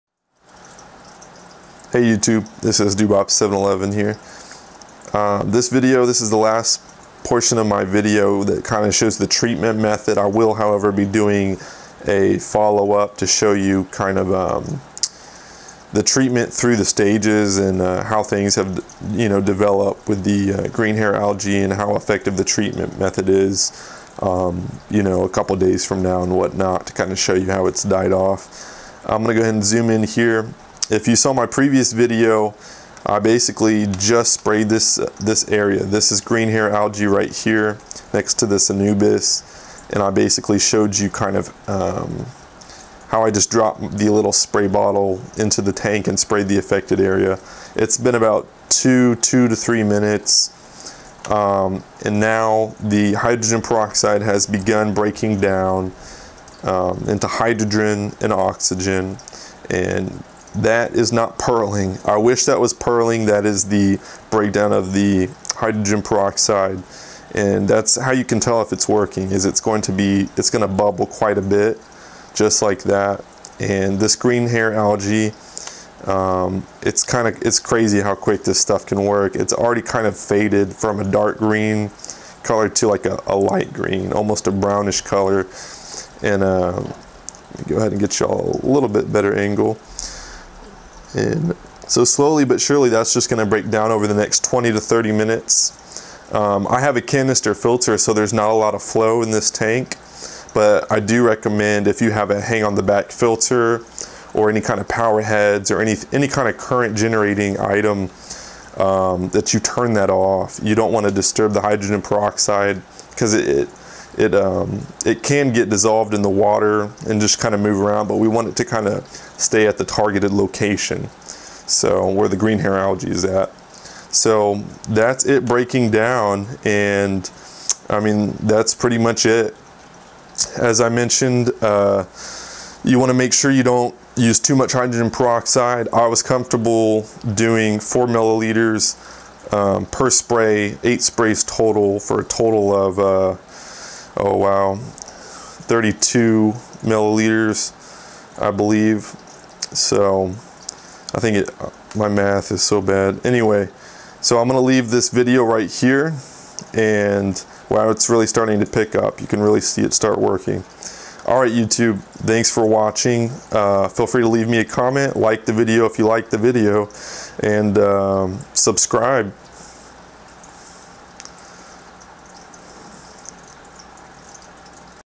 Green Hair-Beard Algae treatment #4
This DIY (do it yourself) instructional and how to video demonstrates how you can treat various kinds of algae such as green hair algae, cyanobacteria, beard algae and other various types of algae found in freshwater planted tanks. While this method can also be used in marine and brackish tanks, this video specifically covers freshwater tanks. Utilizing hydrogen peroxide and a spray bottle I demonstrate how you can spot treat algae in various parts of the aquarium. After spraying selected areas containing algae, the hydrogen peroxide breaks down into hydrogen and oxygen. It's the concentration of oxygen in and around the algae that kills  it. Once the algae is dead it turns a brownish white color.

You can use Seachem Flourish Excel instead of Hydrogen peroxide if your worried about fish/plants.